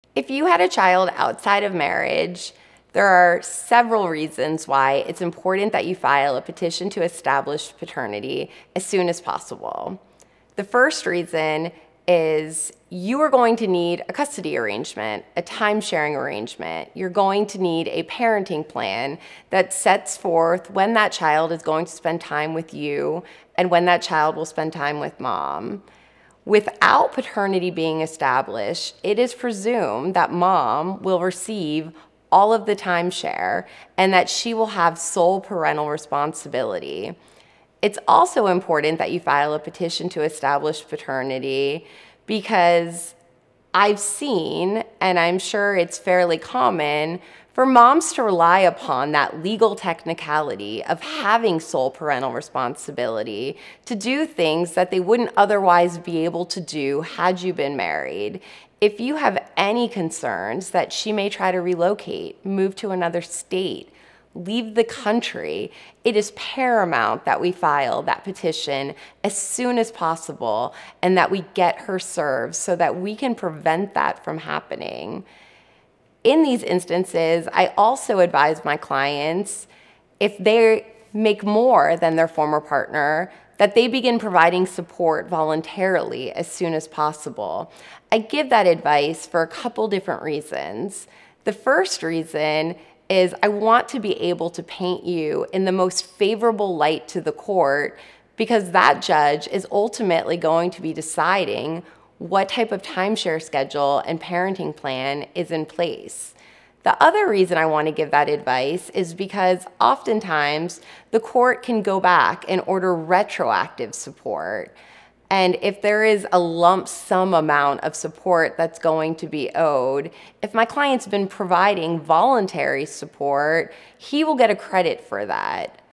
Why Filing for Paternity Early Protects Your Parental Rights
Concerned about losing time with your child if you had them outside of marriage?

Establishing paternity is crucial to securing a parenting plan and protecting your rights as a father.

Don’t wait—filing early can prevent relocation and ensure fair time sharing.
Secure your future with your child by filing today.